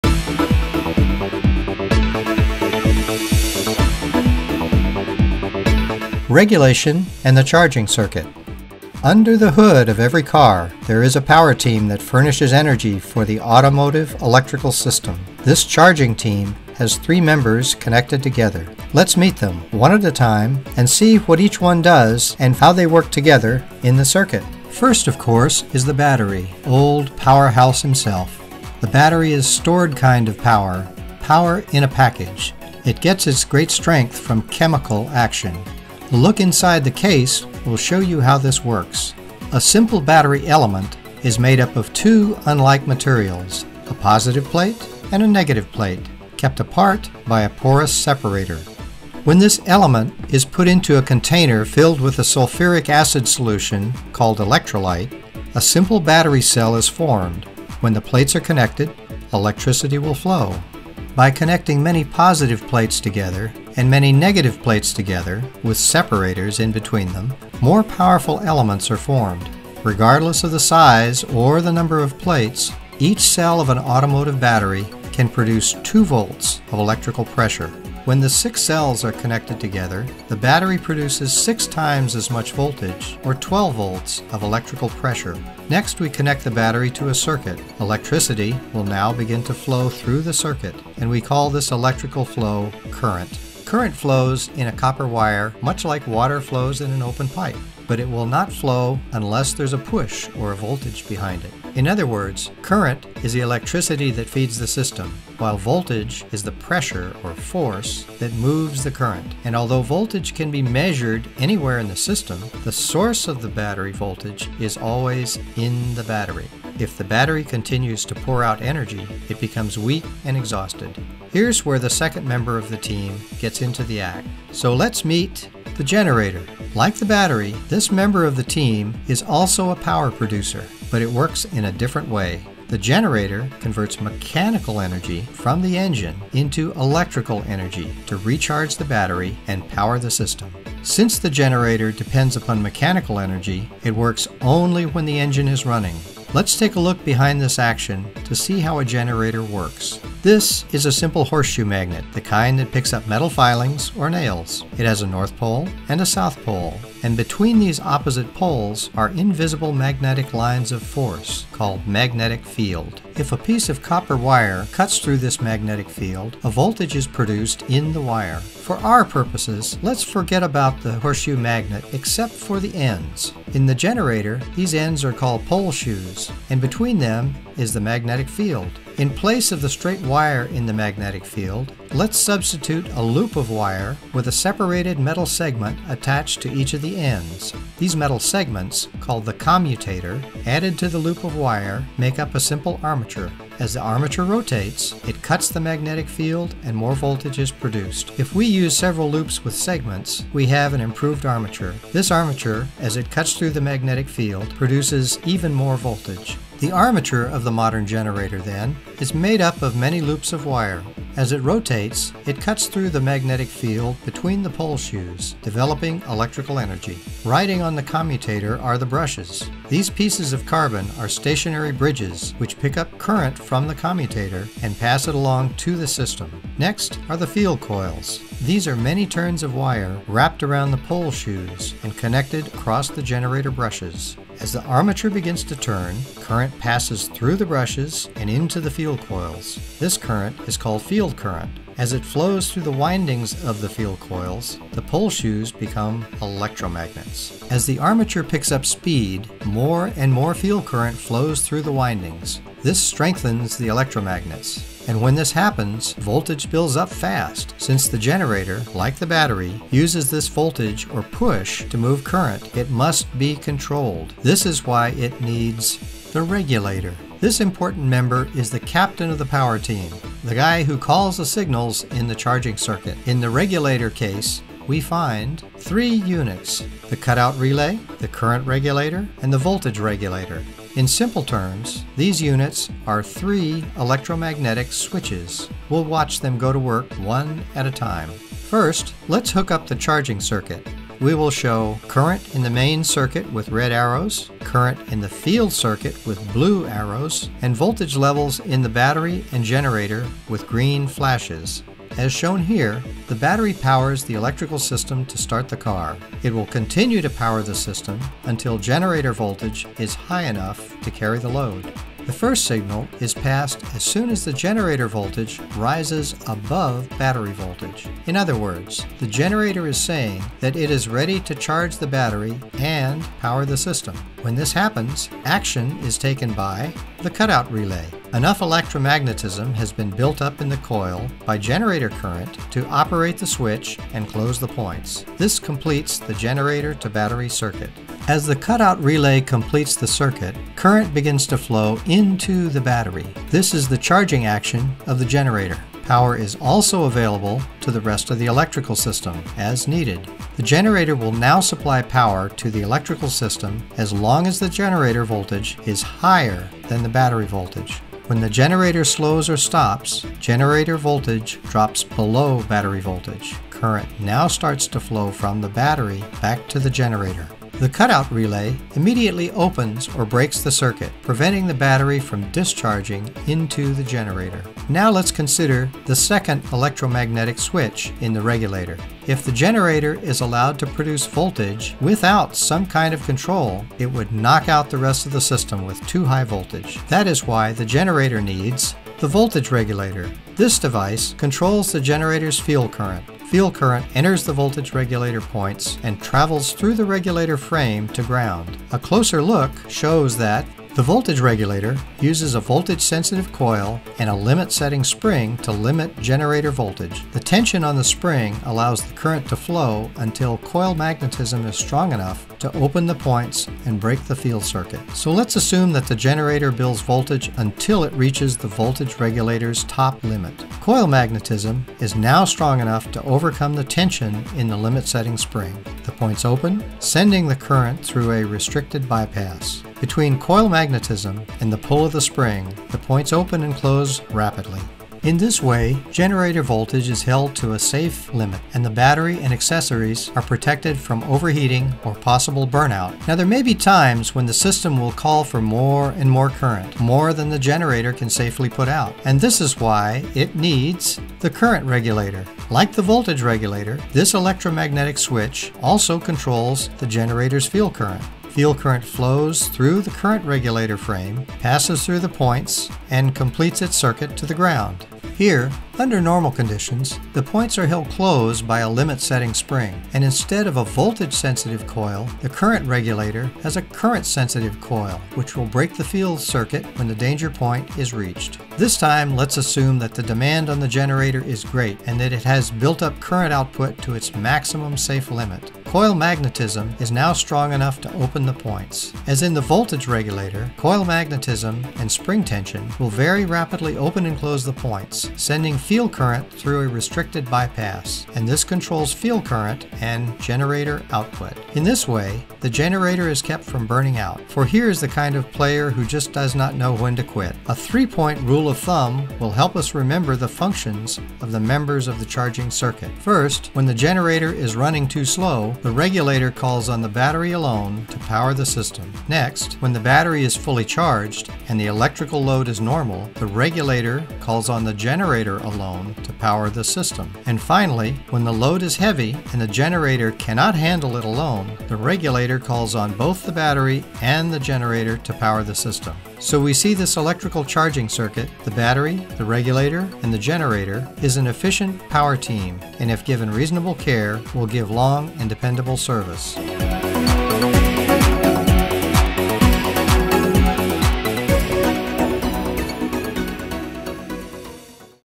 Delco Remy - Regulation and Charging Circuit
This video is was done as a school project. Rob Burgoon did the VO and I did the scoring and video edit. Although this booklet was intended for automotive applications it is just as well suited for aviation applications, the principals are the same.